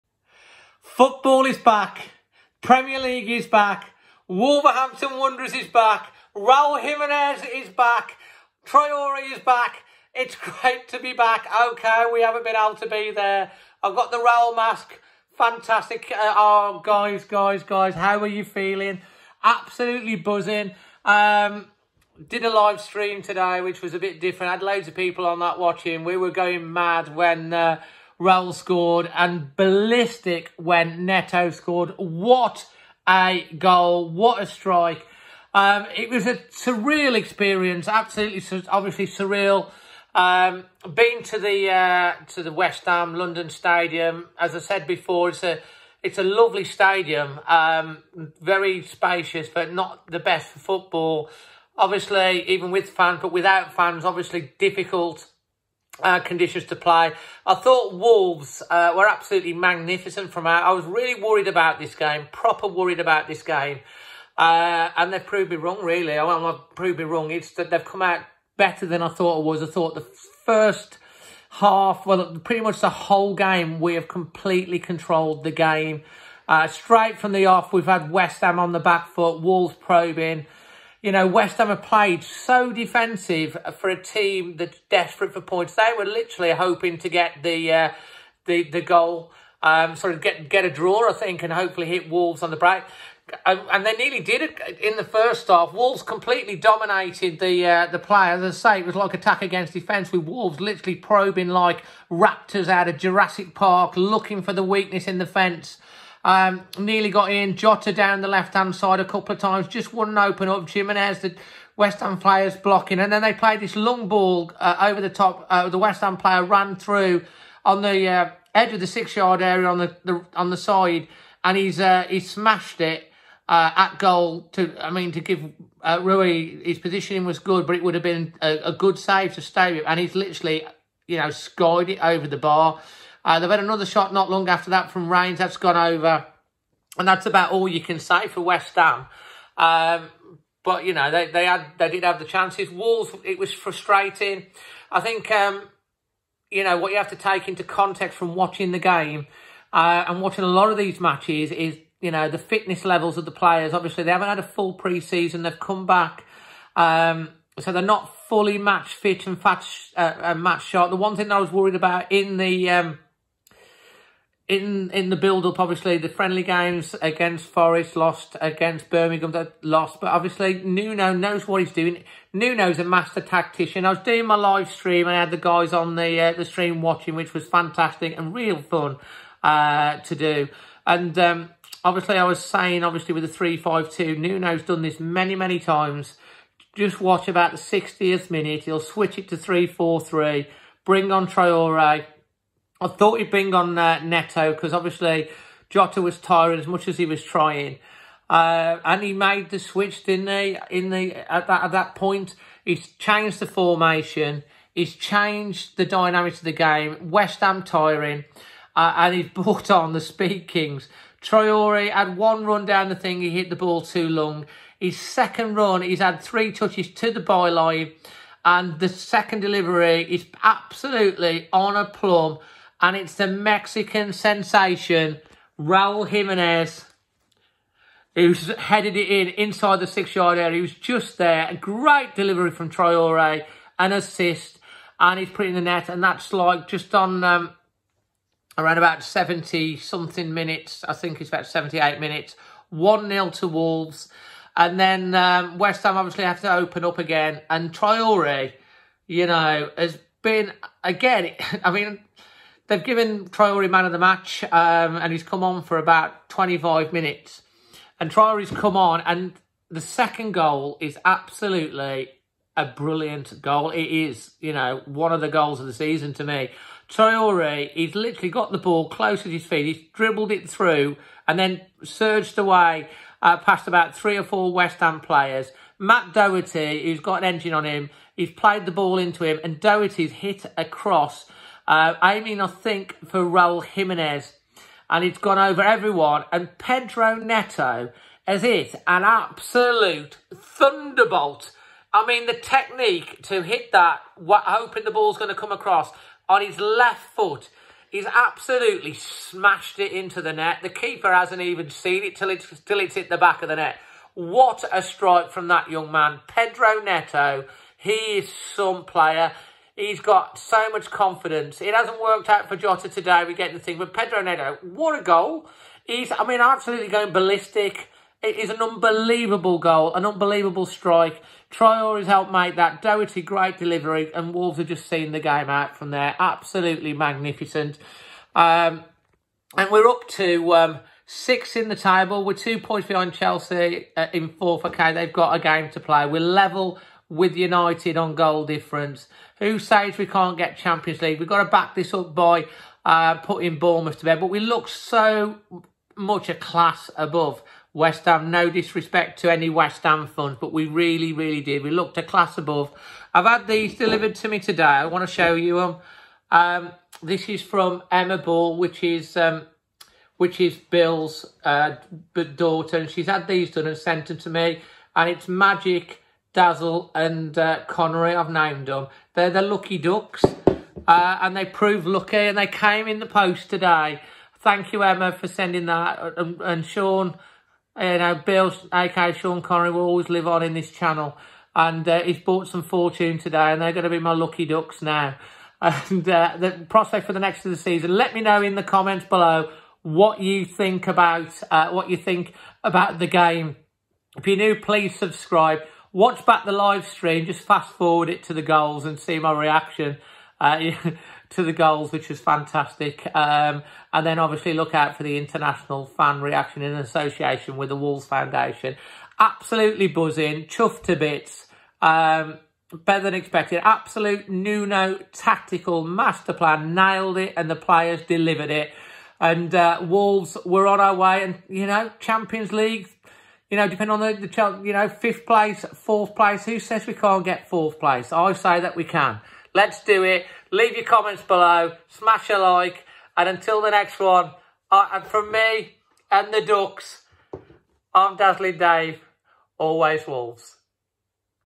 WE’RE BACK 💥 West Ham 0-2 Wolves REACTION 🧡
Reaction as goals from Raul Jimenez & Pedro Neto win the game for Wolverhampton Wanderers 2-0 over West Ham at the London Stadium 🧡🐺

Leave your MOTM and thought ls in the comment section below and check out my reaction to the goals in my live stream 💥